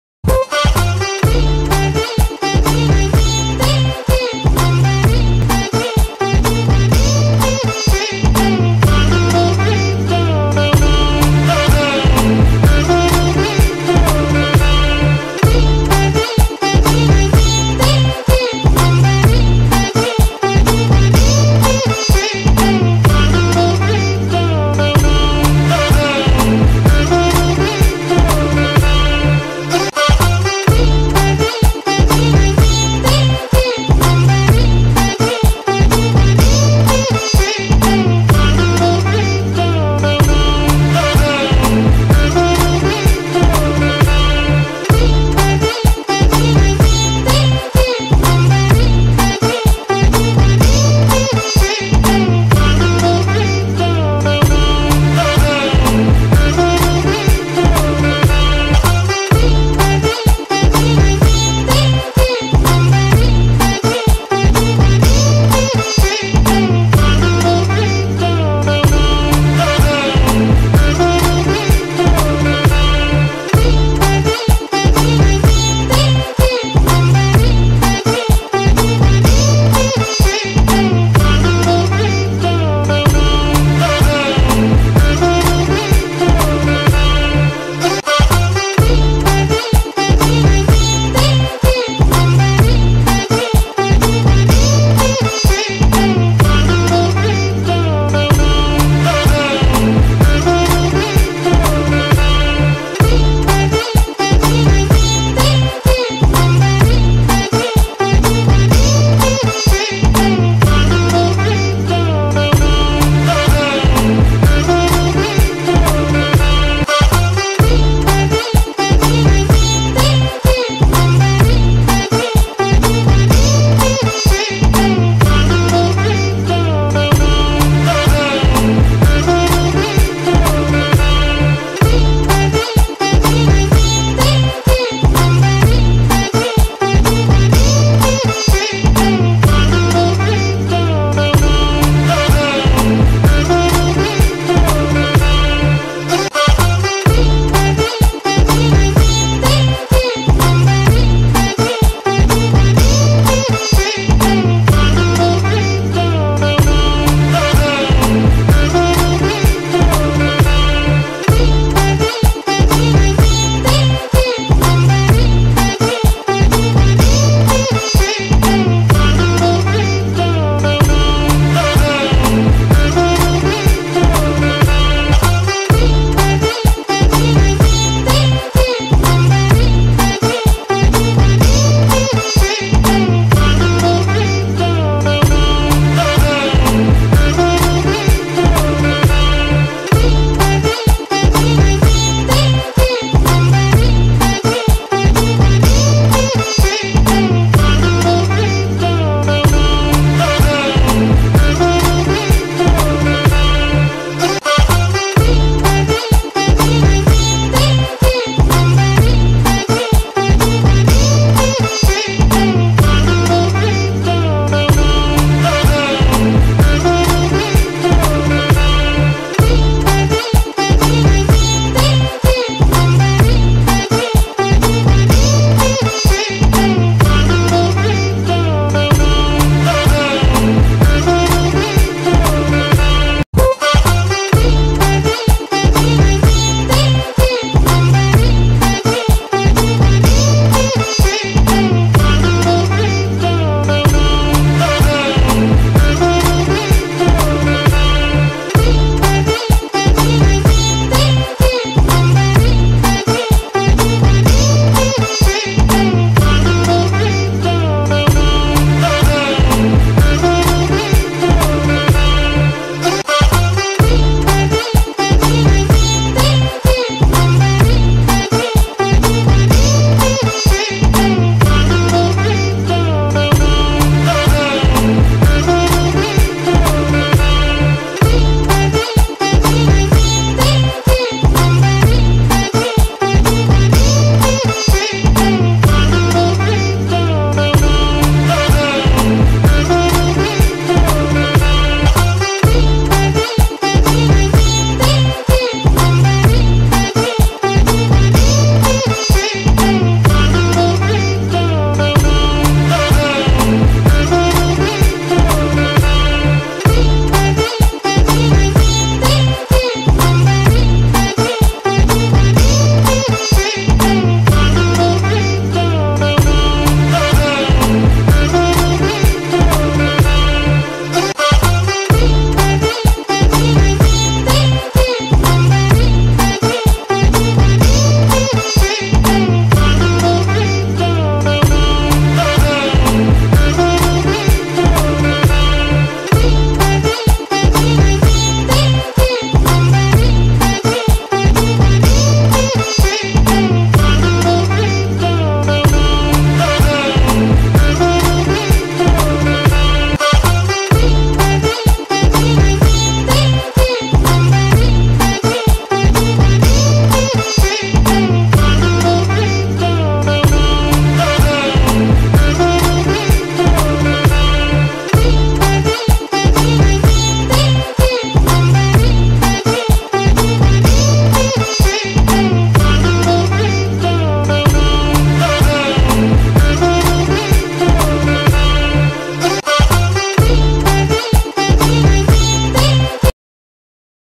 Madhubani online class 6//Madhubani deer painting//how to draw madhubani deer
how to draw madhubani  deer?easy madhubani painting/deer design for cushion/
easy hand drawing deer/best deer drawing

More madhubani drawing tutorials
Madhubani painting or drawing tutorials